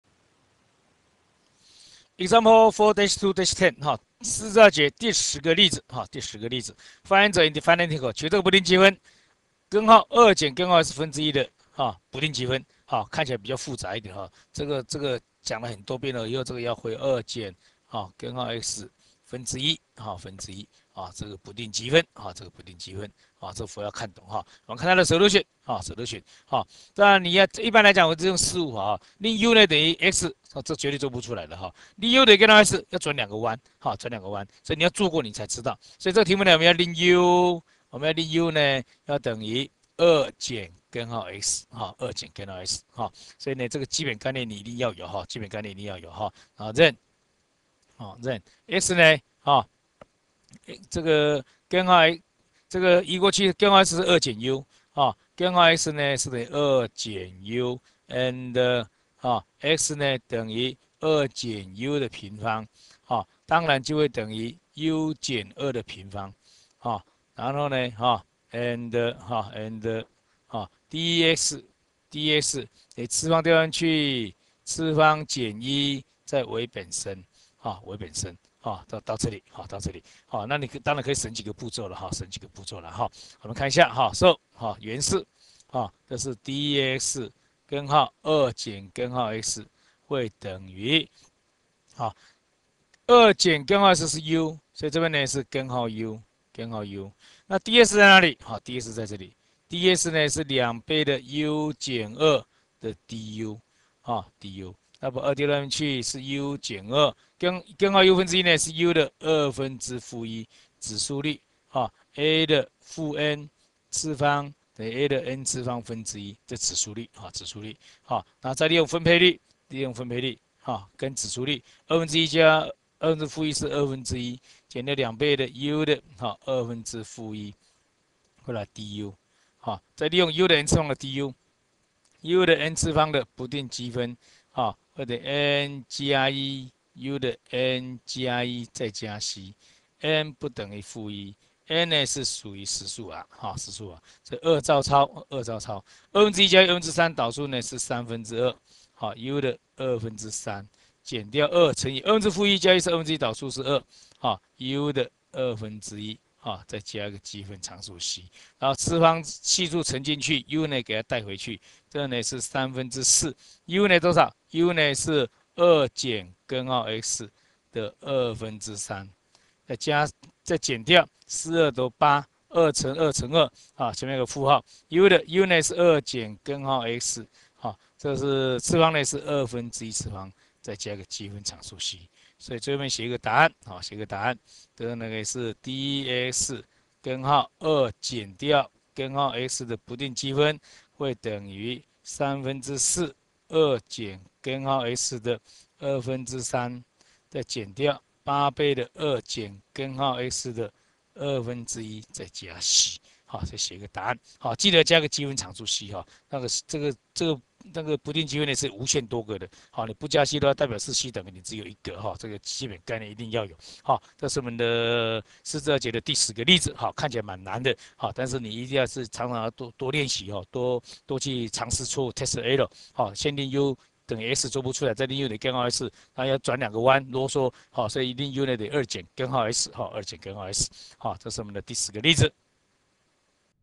Integration by Changing Variables 110 | CH4-2 eCalculus@CSU,Taiwan
Evaluate the indefinite integral int(1/(sqrt(2-sqrt(x)))dx